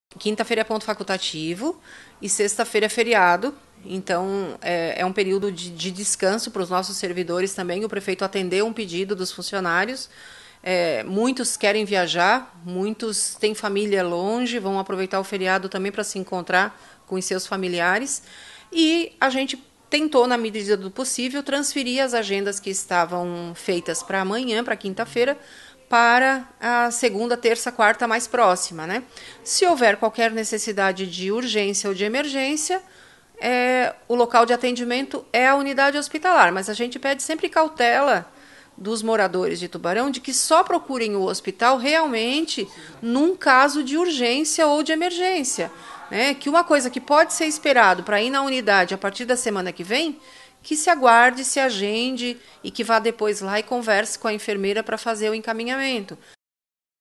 Unidades de Saúde de Tubarão estarão fechadas durante o feriadão
Atenção para quem precisa utilizar os serviços das Unidades de Saúde de Tubarão. Nesta quinta e sexta-feira, elas estarão fechadas. Portanto quem necessitar de serviços de urgência e emergência deve se dirigir ao Hospital Nossa Senhora da Conceição